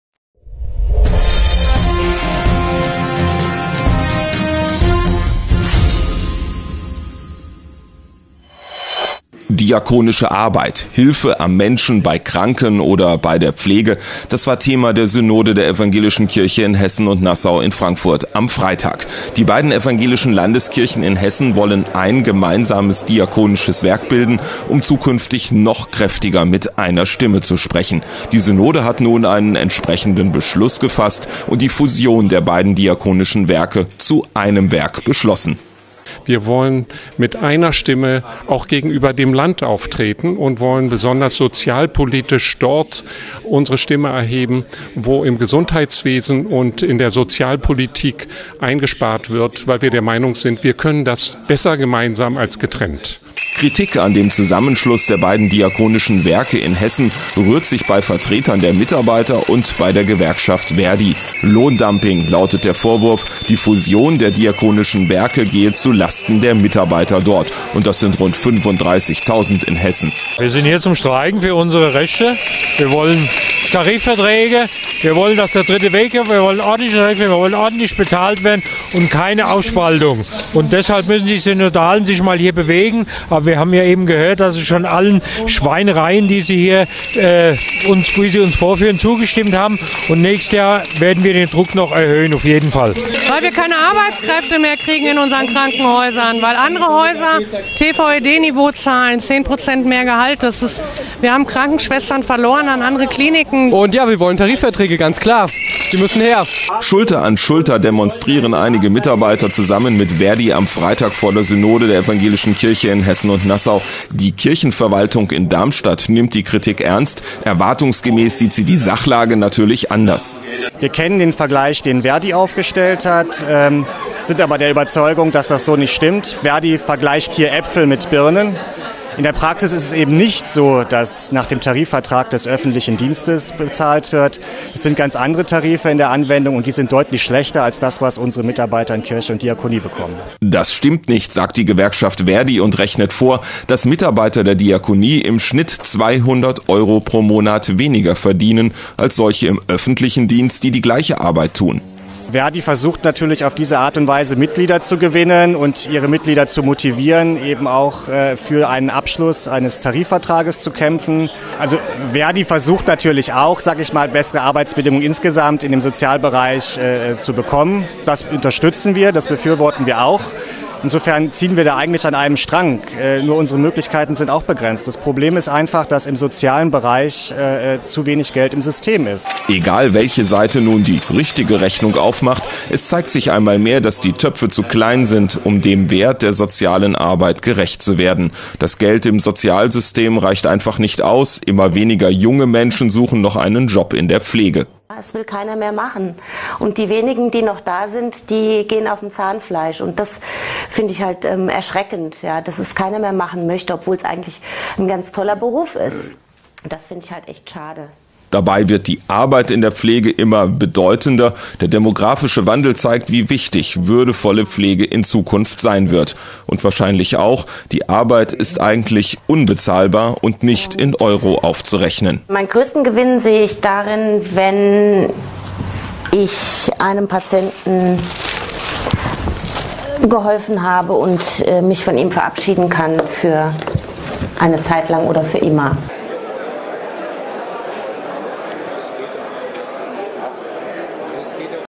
Fusion der Diakonischen Werke beschlossen
Diakonische Arbeit -- Hilfe am Menschen bei Kranken und bei der Pflege -- das war Thema der Synode der Evangelischen Kirche in Hessen und Nassau in Frankfurt am Freitag. Die beiden evangelischen Landeskirchen in Hessen wollen ein gemeinsames Diakonisches Werk bilden, um zukünftig noch kräftiger mit einer Stimme zu sprechen. Die Synode hat nun einen entsprechenden Beschluss gefasst und die Fusion der beiden Diakonischen Werke zu einem beschlossen.